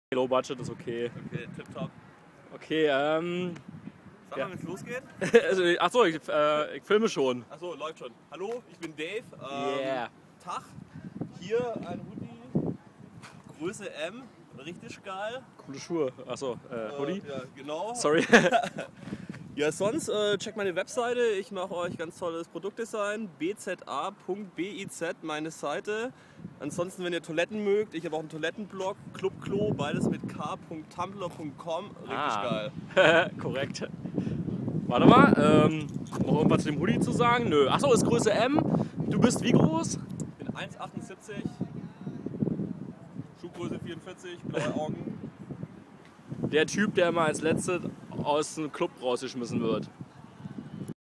YACKFOU Men Hoody M David Bizer
Dave, der alte Schwabe trägt hier einen YACKFOU Unisex Hoody in der Größe M mit dem Motiv »Cassipoeia« Der Produktdesigner macht nict nur schicke kleine Accessoires, sondern kocht auch gern und gut. Dave, thumbs up!